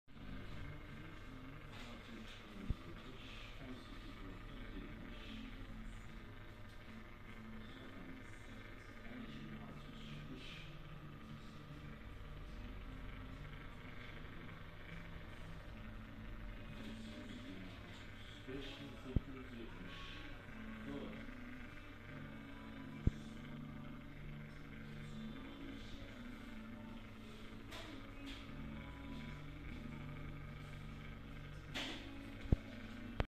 ALTINOFİSİ 4mm Alyans 14 Ayar Altın HD VİDEOLU BİZİ TAKİP EDİN 34
NQ:40

Altın Ayar: 14 Ayar 0,585 Milyem

Genişlik: 4 mm

Ağırlık:  1,55gr

Üretici:  Acar

Üretim Şekli: İçi boş

Sertifika: TC. Maliyesi Ayrıntılı Resmi Faturası

Yeni modeler için web sitemize bakınız